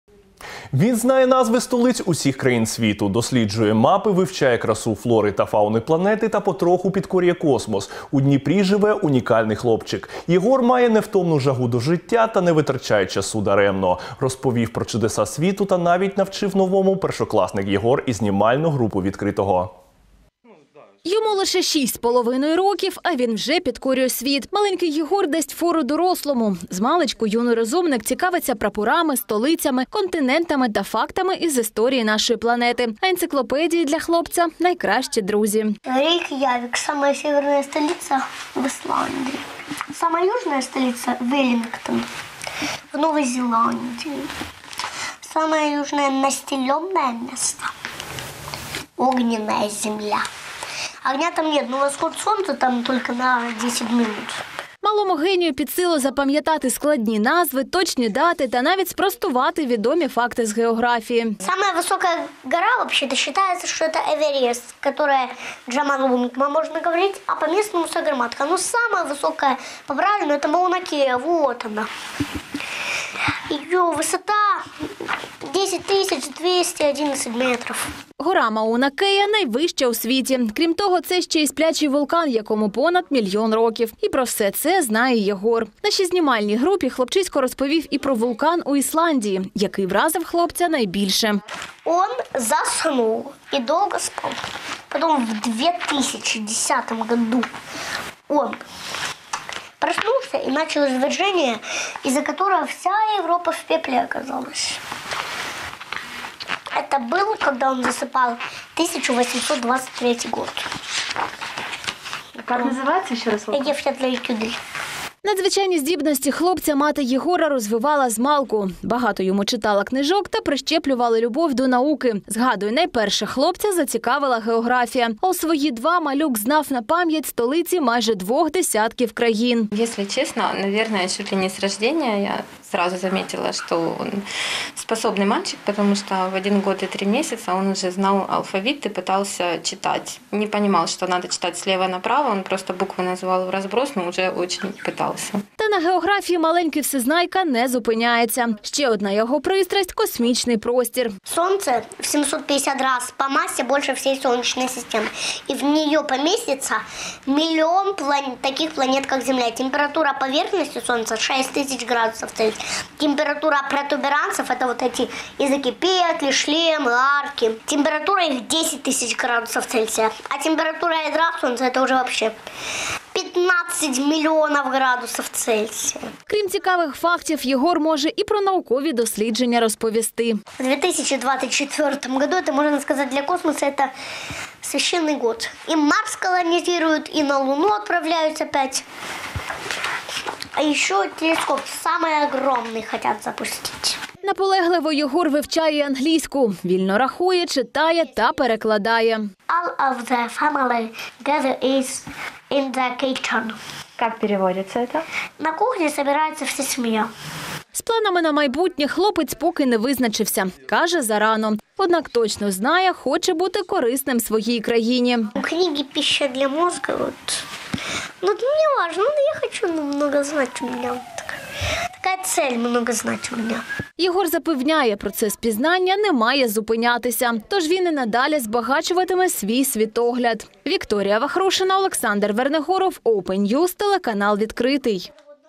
Із жагою до знань. У Дніпрі живе маленький вундеркінд
Він знає назви столиць усіх країн світу, досліджує мапи, вивчає красу флори та фауни планети та потроху підкорює космос. У Дніпрі живе унікальний хлопчик. Єгор має невтомну жагу до життя та не витрачає часу даремно.